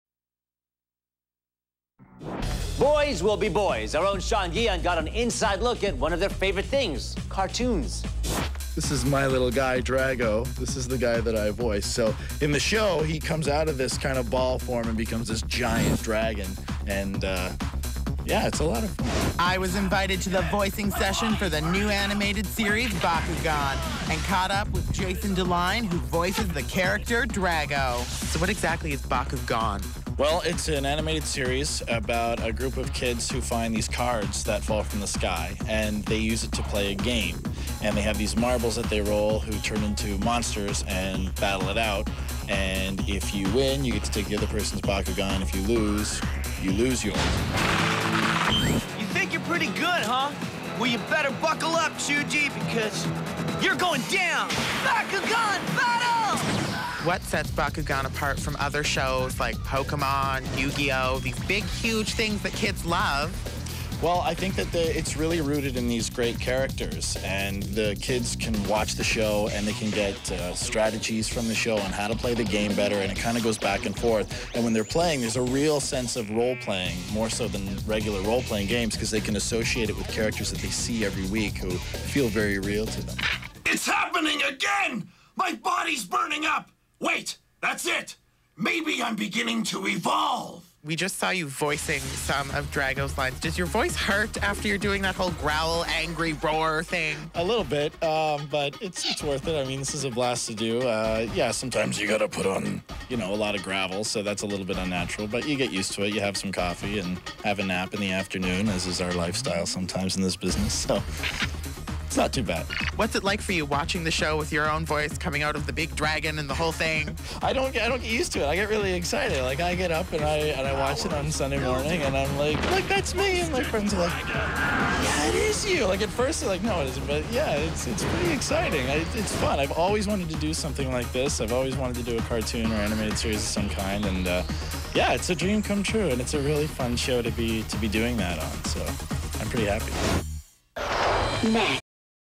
Jason Deline's Star!TV interview for Bakugan Battle Brawlers
On Tuesday, September 4th, 2007, Jason Deline was interview on Star! Daily about Bakugan Battle Brawlers, a kids' animated series in which I voice the character Drago.